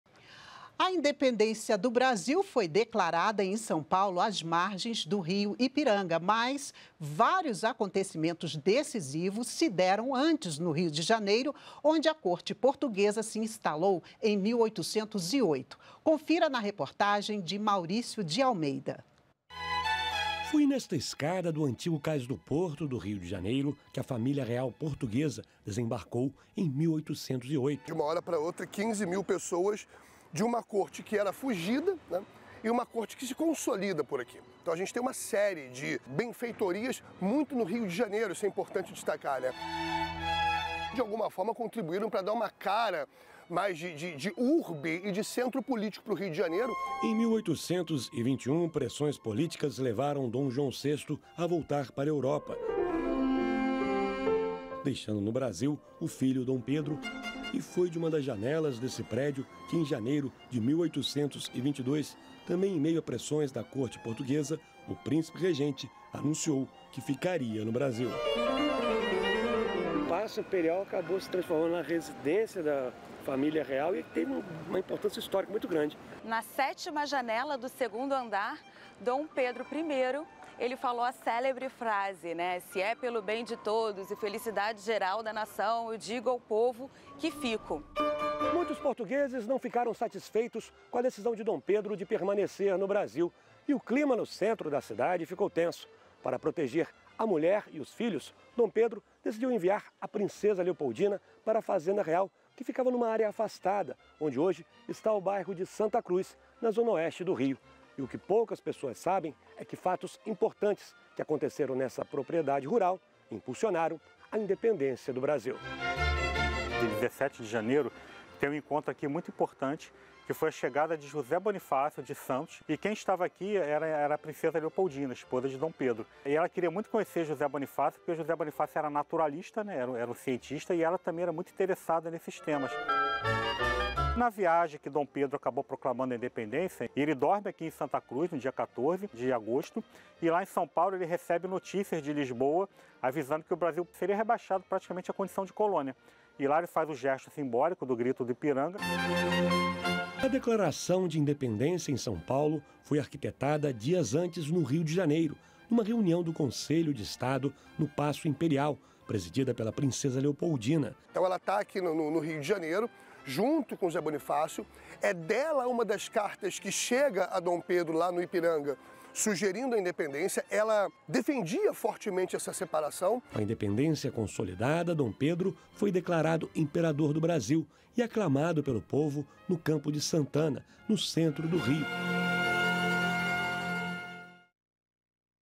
Bicentenário da Independência | Acontecimentos decisivos no Rio de Janeiro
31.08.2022 - A independência do Brasil foi declarada em São Paulo (SP), às margens do Rio Ipiranga. Mas vários acontecimentos decisivos ocorreram antes no Rio de Janeiro, onde a corte portuguesa se instalou em 1808. Confira na reportagem de Mauricio de Almeida.